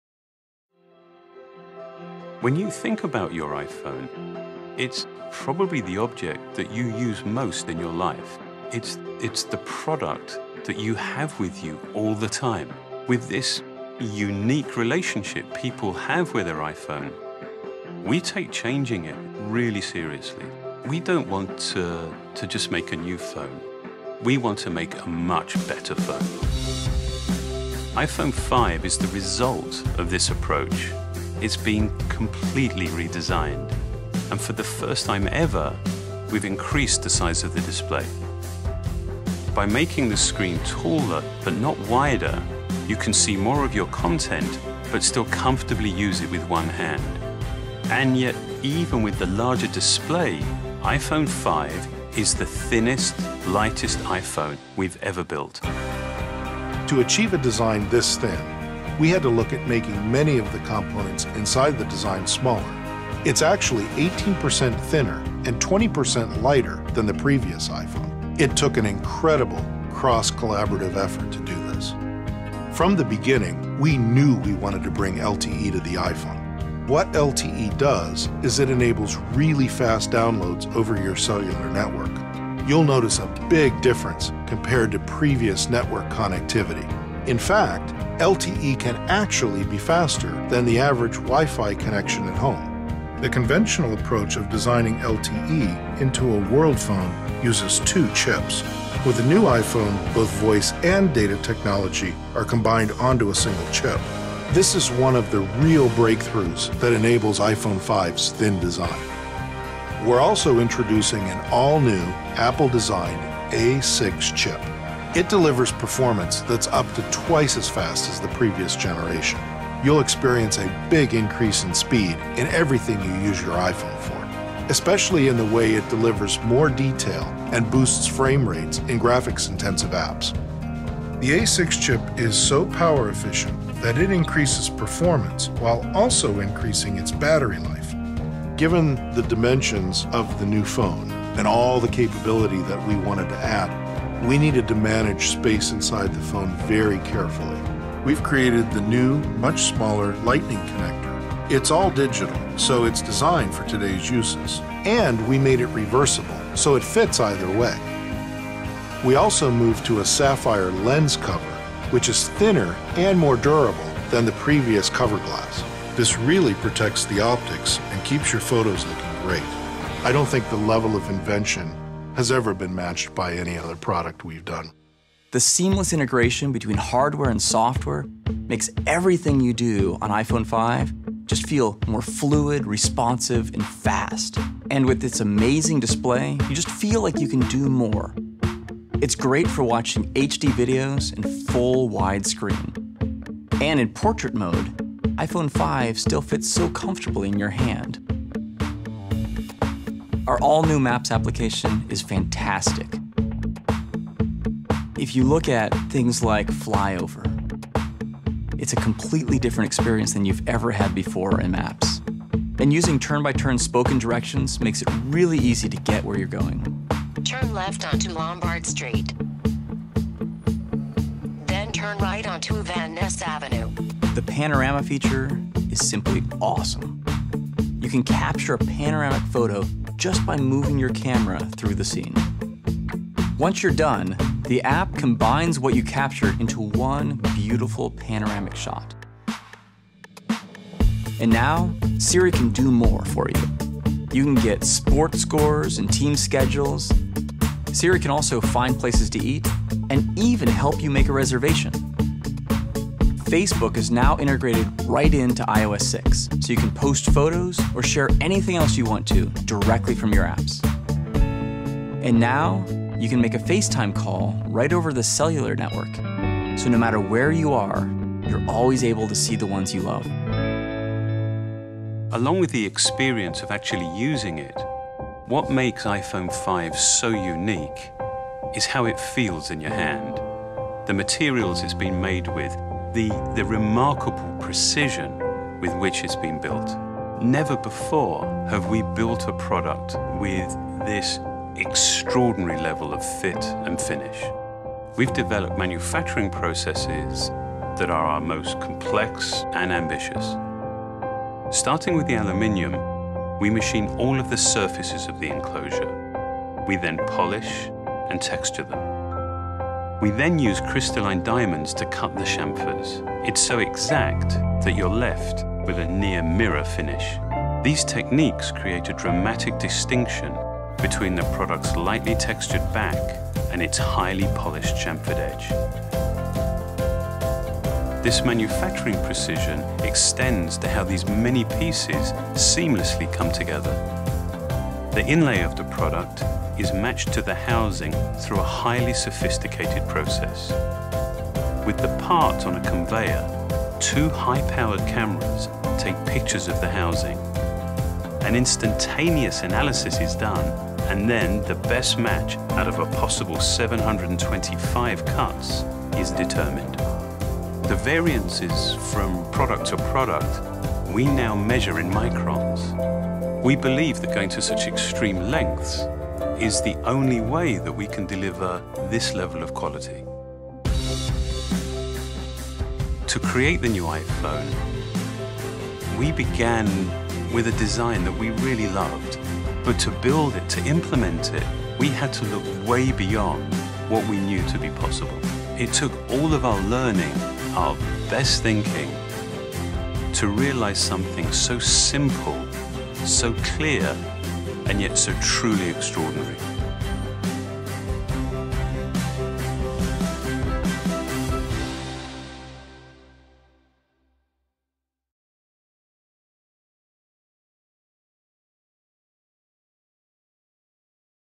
iPhone 5 Official Trailer - Apple 2012 [HD]
Apple - iPhone 5 - The thinnest, lightest, fastest iPhone ever.

A remarkably slim design that still makes room for a larger display and a faster chip. Ultrafast wireless that doesn't sacrifice battery life. And all-new headphones designed to sound great and fit comfortably. So much went into this iPhone. So you could get even more out of it.

Pre-order online starting September 14.

Or buy at your favorite Apple Retail Store starting at 8:00 a.m. on September 21.